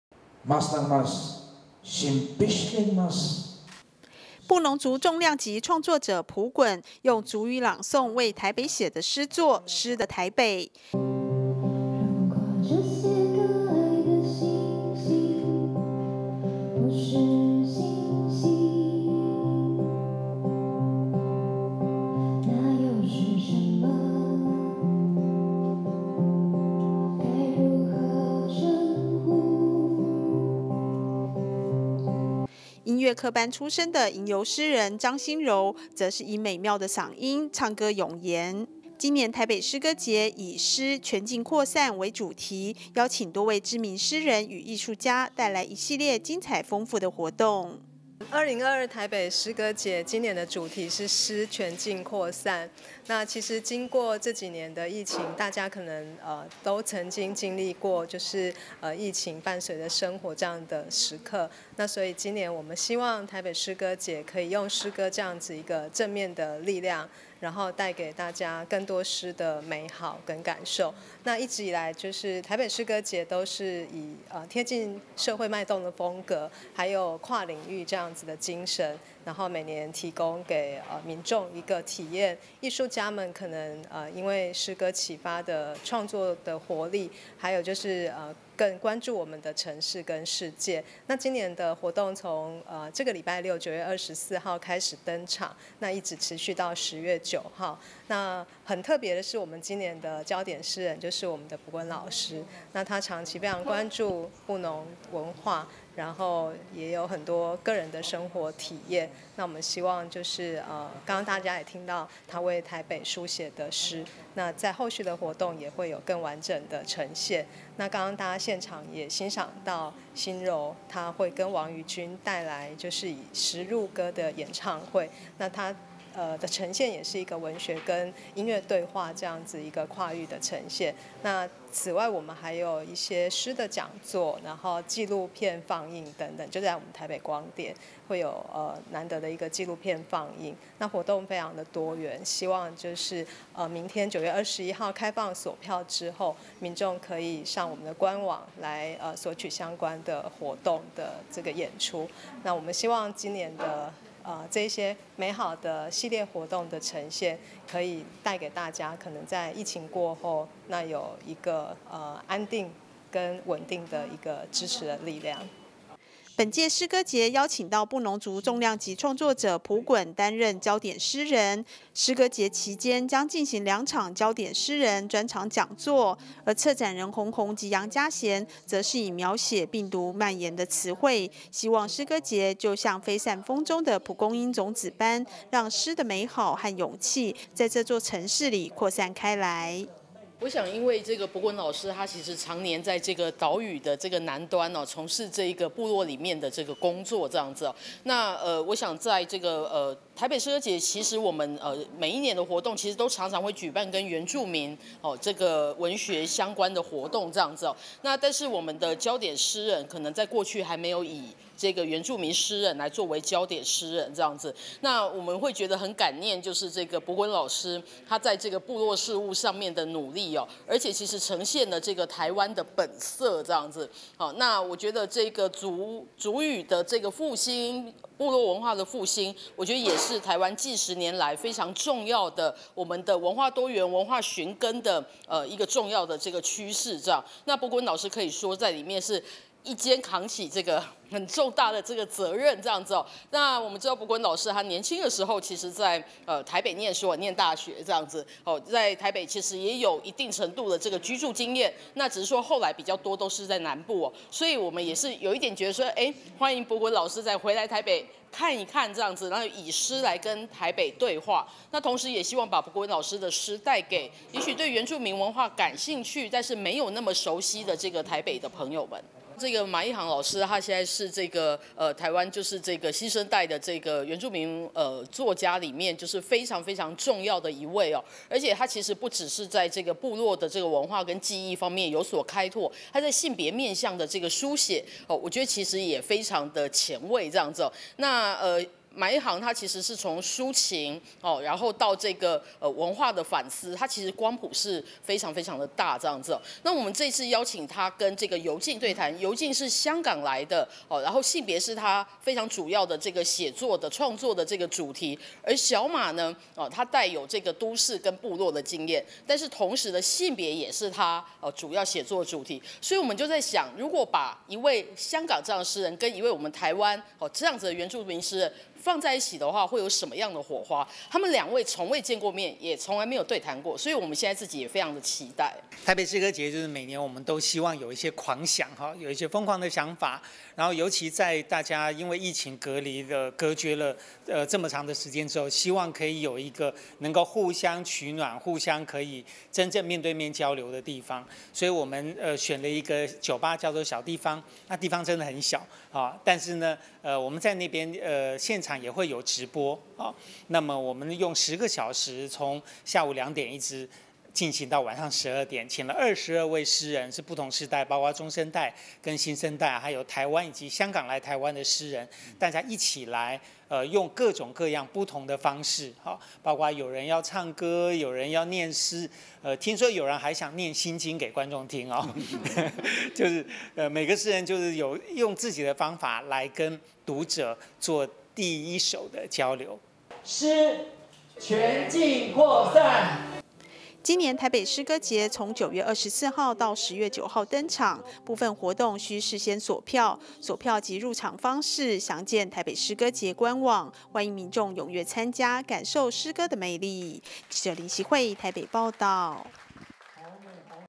北市 藝文|台北詩歌節 9/24起讓《詩，全境擴散》
邁入第22屆的「臺北詩歌節」，今年以「詩，全境擴散」為主題，呼應後疫情時代，心靈療癒的渴求與想望。邀請多位知名詩人與藝術家，帶來開幕詩演出、創作演唱會、詩人講座、讀詩馬拉松等豐富精彩活動，透過詩歌帶領大家飛越界限，體驗詩意和想像。